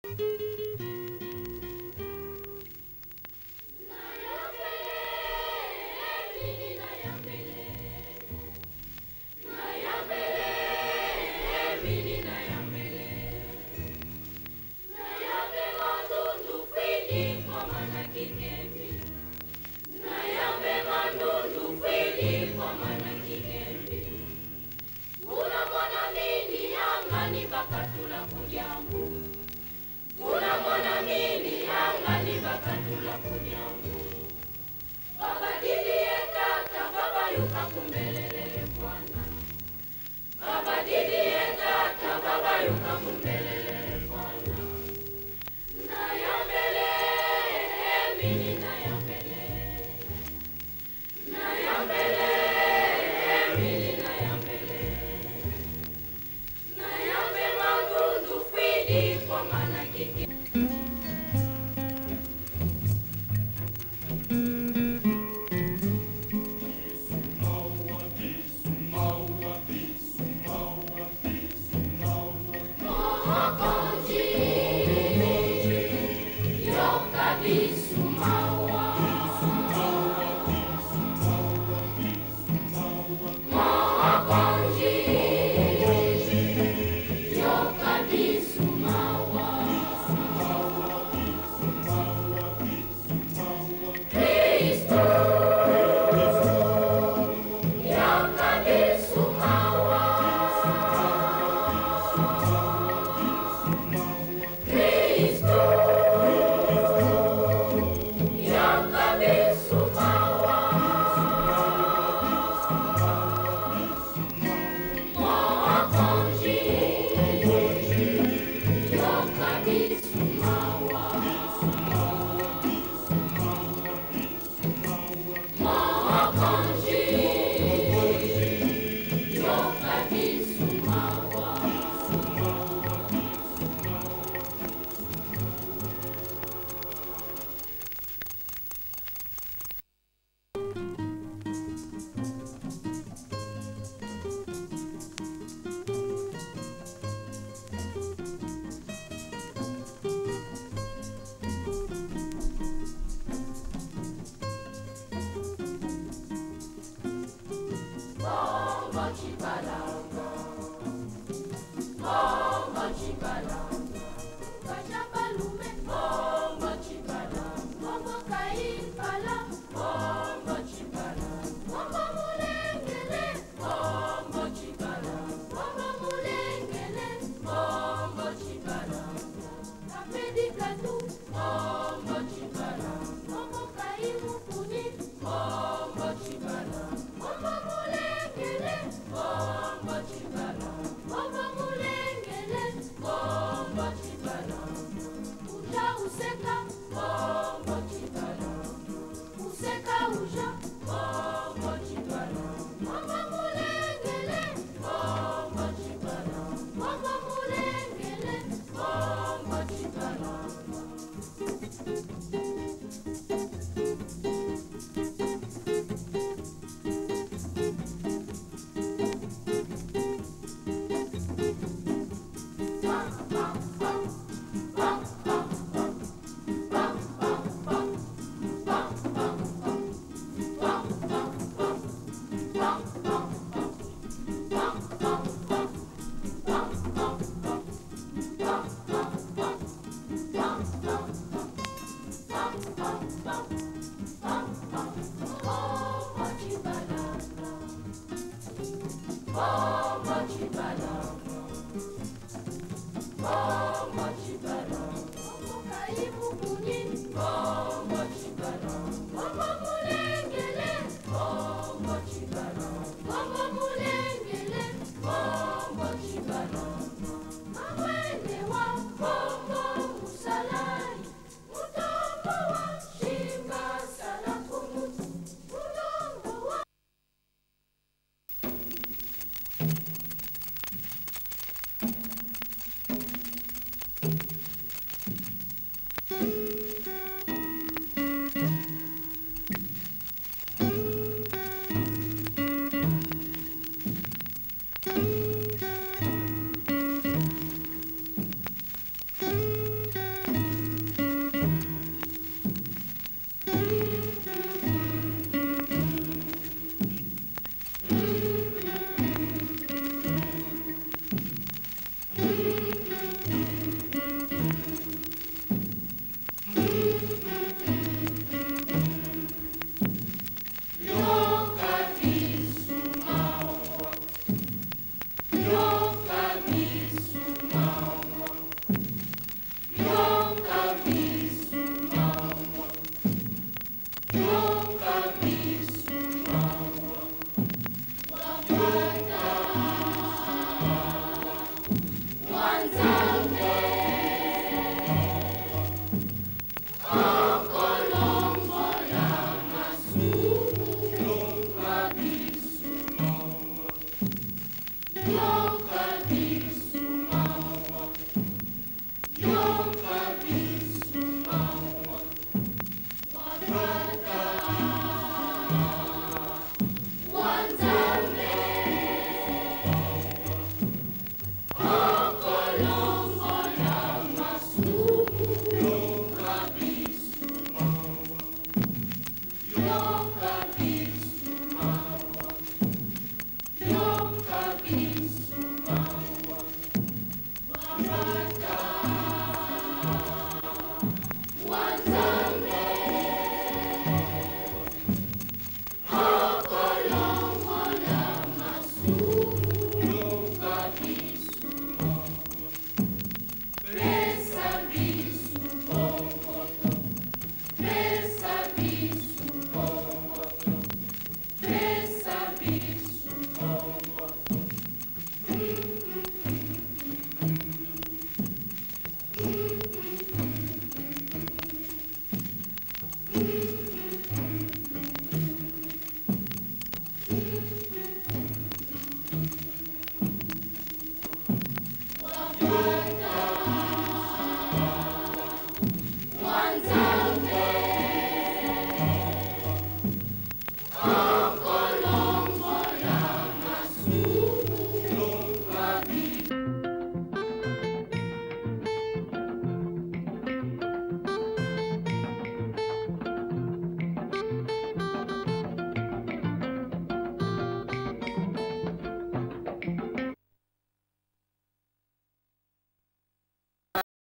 Chem Chem Yetu,seul film de ce groupe réalisé à la RENAPEC(direction:Bernard Van Den Boom)
Chem Chem Yetu,chorale des jeunes de Kinshasa,sous la direction de Bernard Van Den Boom,avant la tournée européenne de 1973.
Film réalisé à la renapec